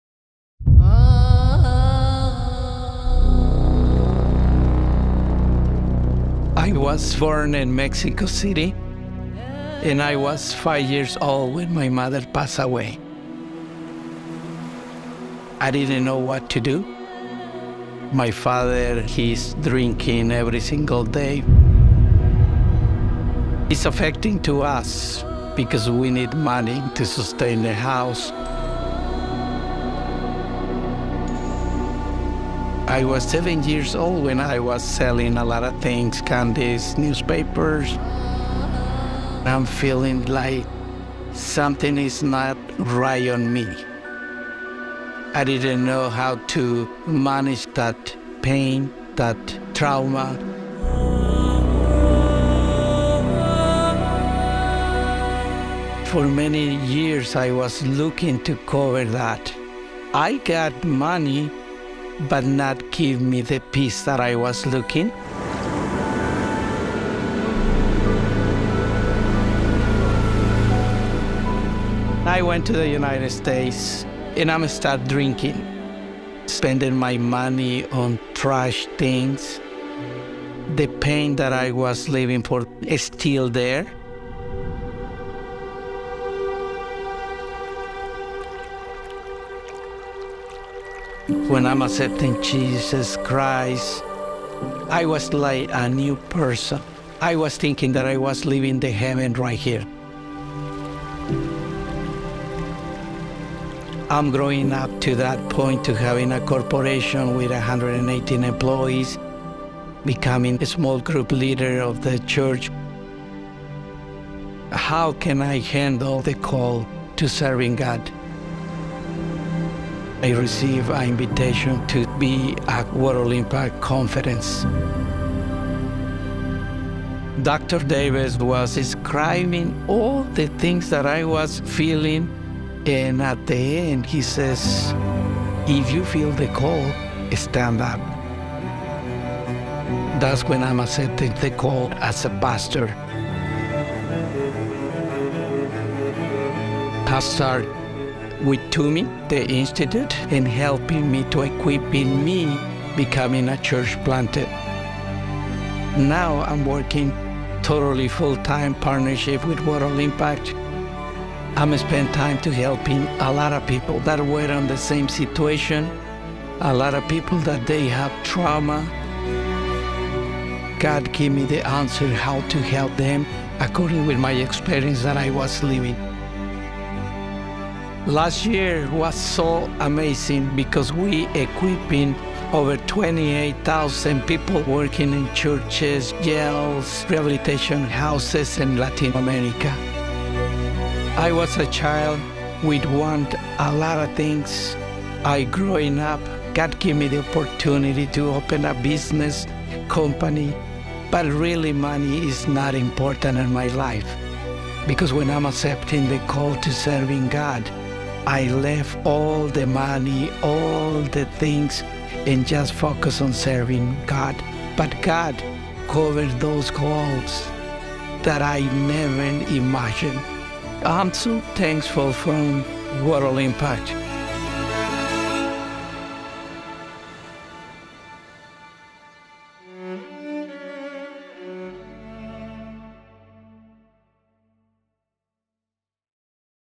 David's Story: Spreading the Gospel in a Restricted Country
David is a passionate believer and leads small groups and a preaching ministry in his local church. Together with World Impact, David also leads an international ministry that has planted seeds of hope and transformation in lives and communities across Latin America.

He has trained more than 20,000 leaders and equipped over 100,000 people with Bible-based materials.

One of our ministry developers, Juan Pablo, befriended David at his lowest point. Juan Pablo invited David to the church World Impact helped him start, and he soon accepted Jesus Christ as his Savior.

Juan Pablo encouraged David to enroll in World Impact’s Church-Based Seminary program, and David not only embraced what he learned but also attended a World Impact evangelism conference. There, he heard The Urban Ministry Institute founder, Dr. Don Davis, speak about the high calling of full-time missions work.

That’s when he accepted the call as a pastor.